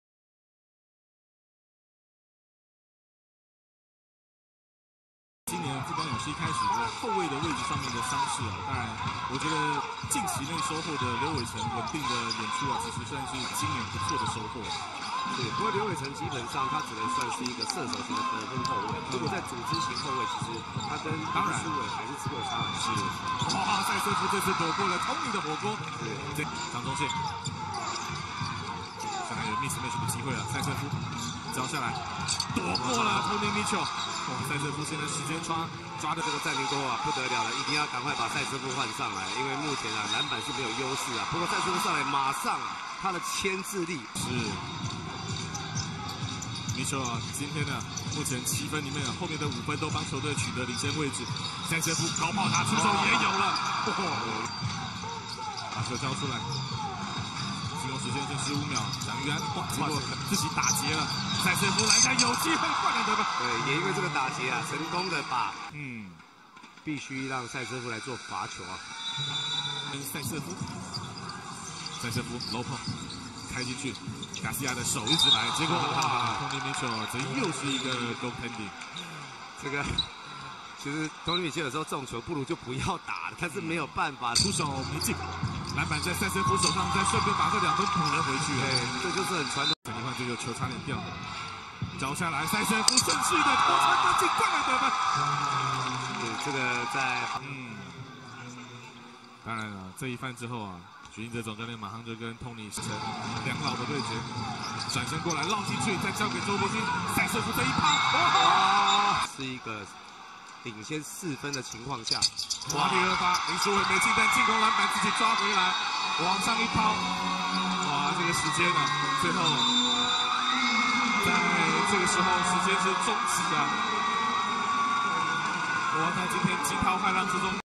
[SBL] 2019/03/29 Ihor Zaytsev 21pts 10rb Full Highlights vs 富邦
2019/03/29 Ihor Zaytsev 21pts 10rb Full Highlights vs 富邦

影片來源 : 麥卡貝、WINTV
視頻都經過編輯，無侵犯版權之意。
如有侵權，將移除內容！